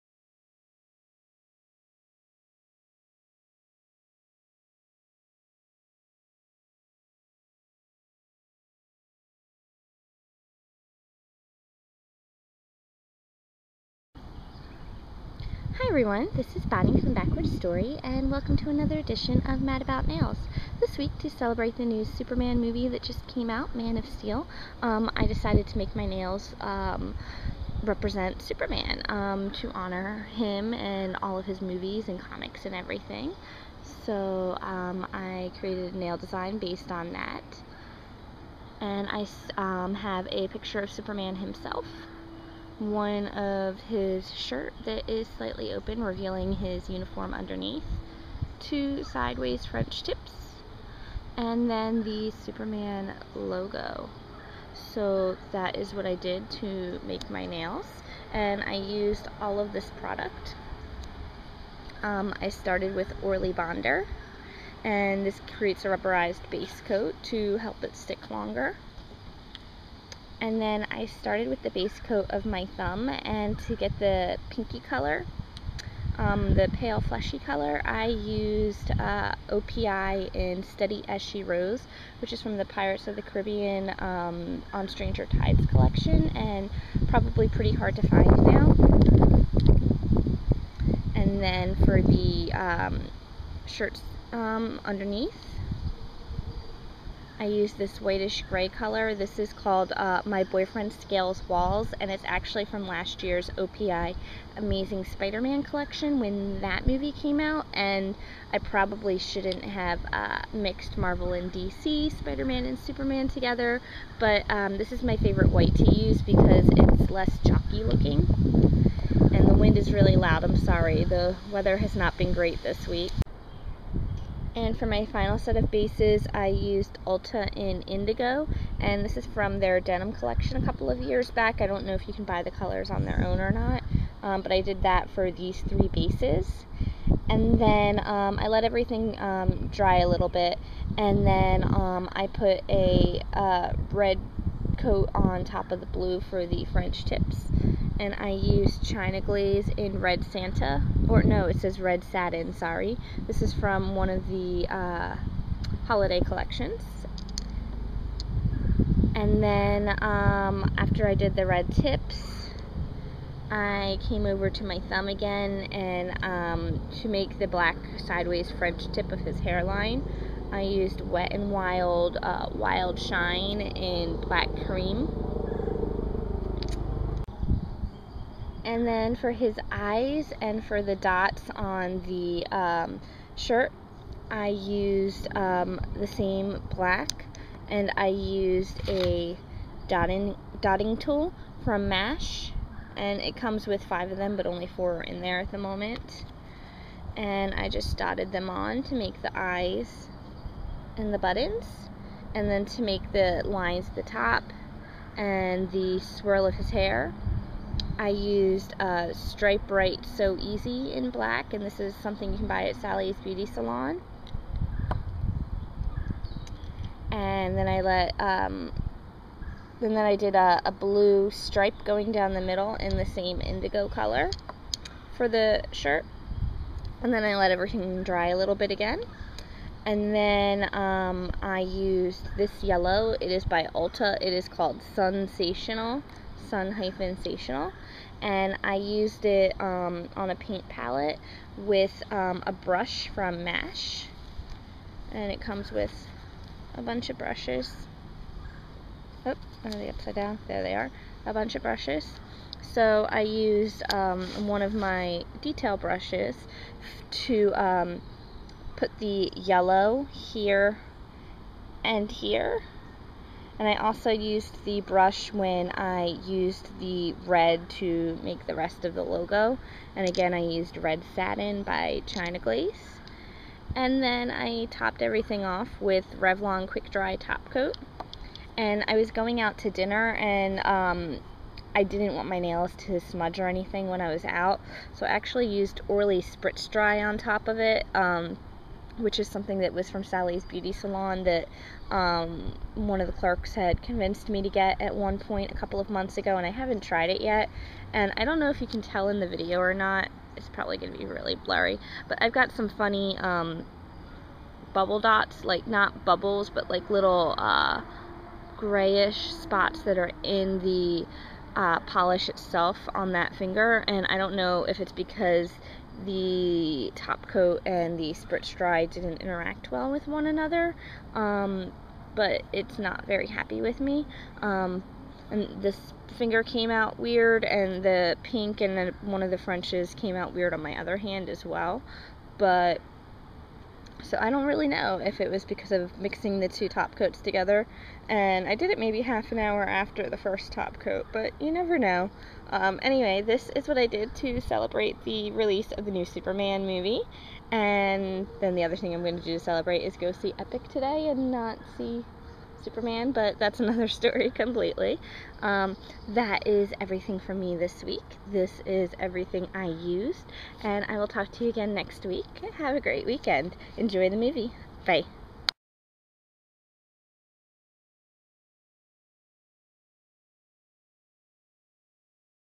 Mad About Nails: Superman Nails
To celebrate the release of MAN OF STEEL, I decided to feature Superman on my nails this week!